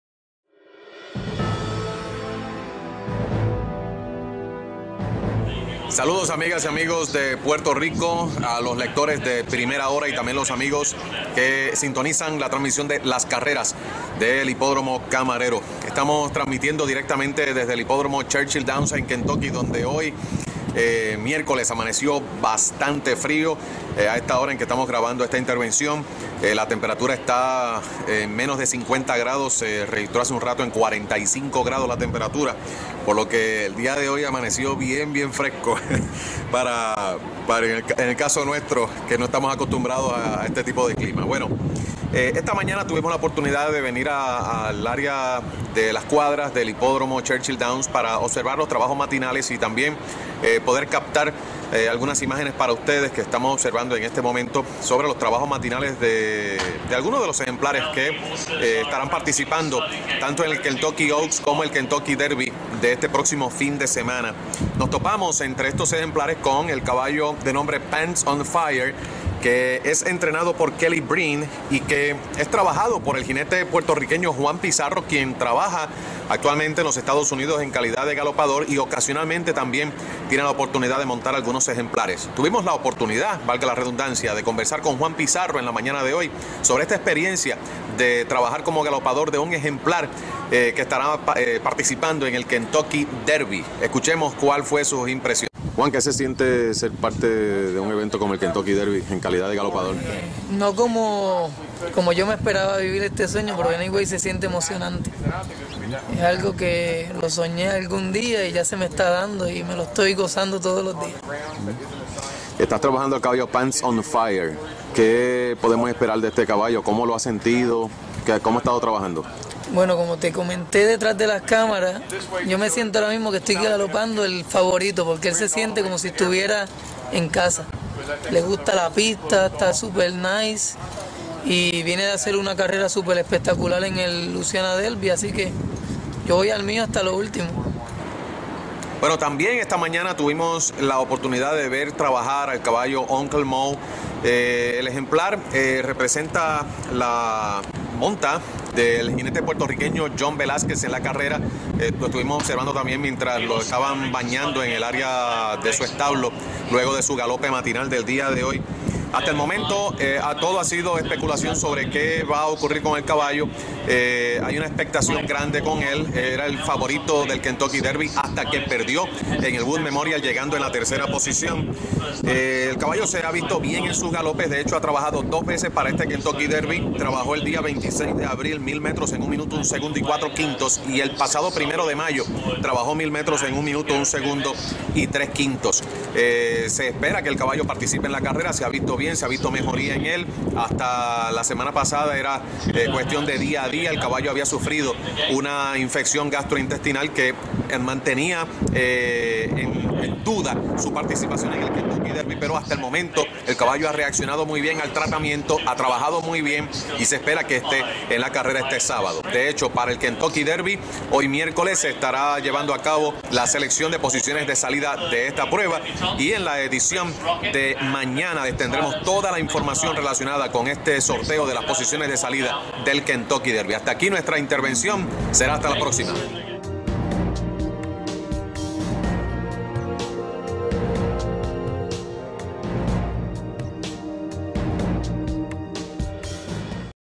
Kentucky Derby 2011 (Joe Bruno Coverage) / Wednesday
Showing the workouts of Uncle Mo and Pants on Fire of Wednesday on Kentucky.  Producido por Joe Bruno y editado por Alberto Xtreme